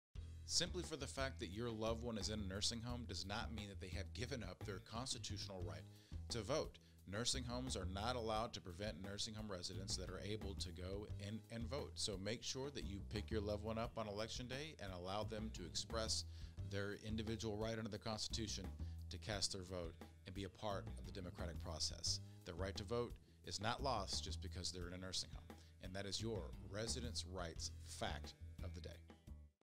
Resident's Right Fact 2: Right to vote
Please call for a free consultation with an attorney if your loved one has been seriously injured or abused at a Georgia nursing home.

Resident's Right Fact 2: Every resident retains the right to vote! The nursing home is not allowed to make it  difficult for the resident to assert his or her right to vote!